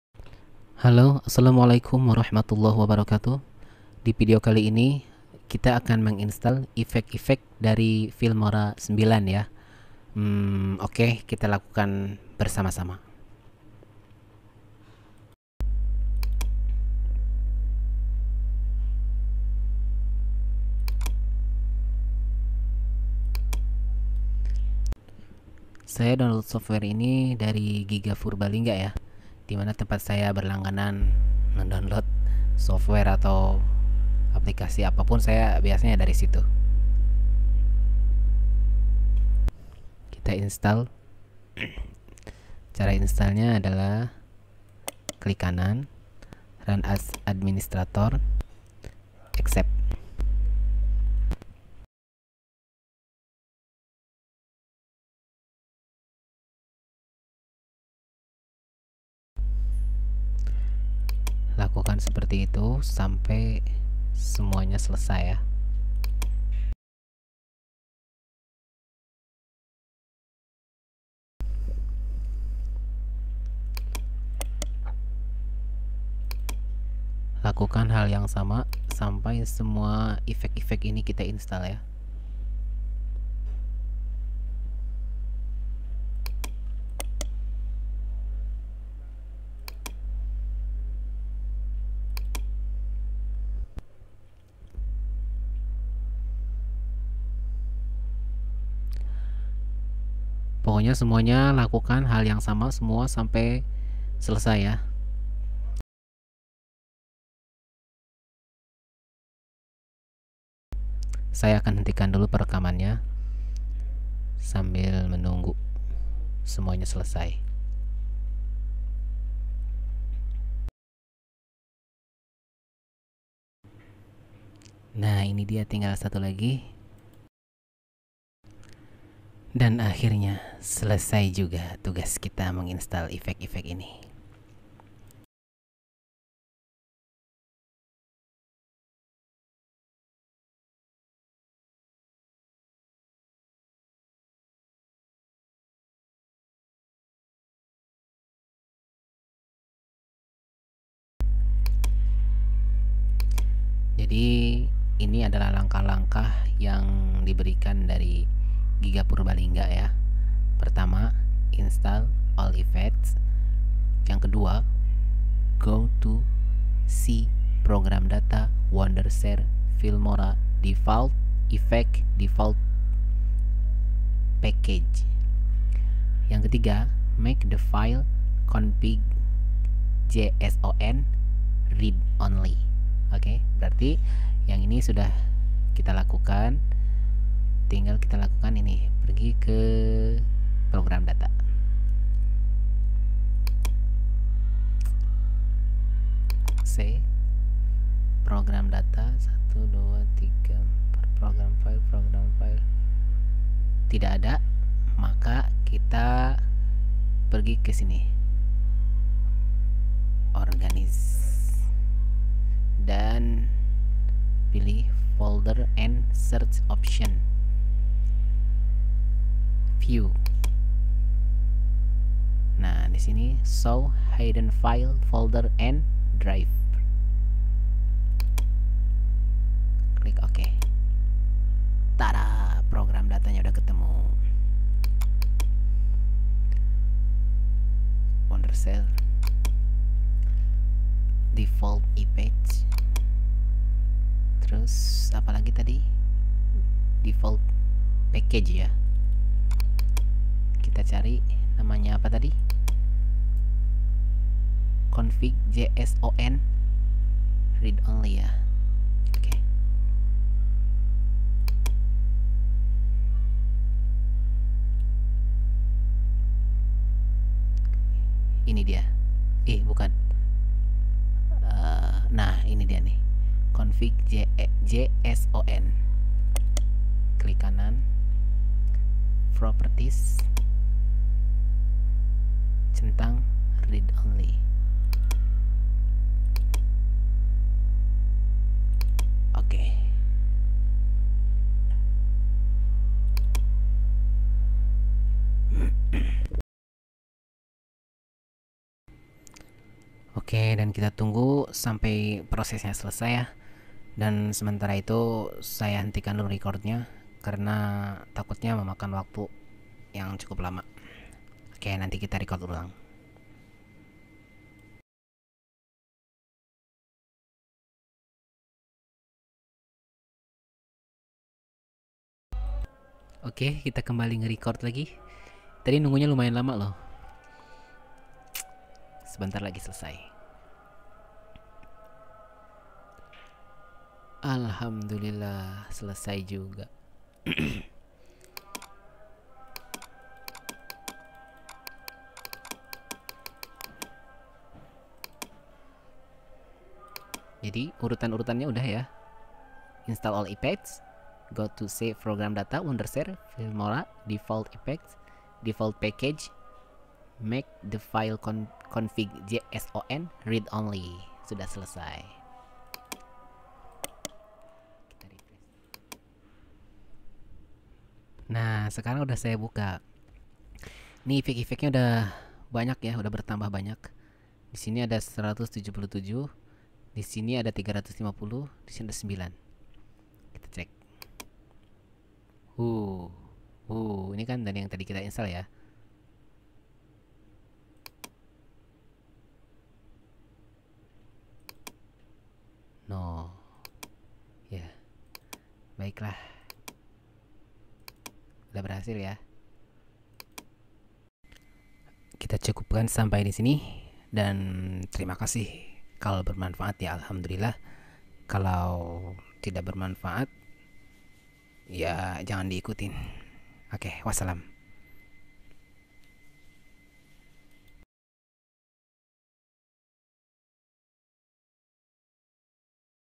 Cara Install Effects Filmora9
di video ini adalah cara instal effects effects filmora9
yang masih bingung silahkan ikuti cara cara di video ini
tapi hrus sabar nyimak nya ya,,,, sayya sudah sebeusaha mungkin untuk membuatnya jelas. denga windows 7 ultimate.. sebenarnya laptop saya berat pakai ini jadi setelah bikin tutorial ini, bakalan saya unistall.

oya ini link efffects filmora 9 nya ya.

filenya milik mas gigapurbalingga.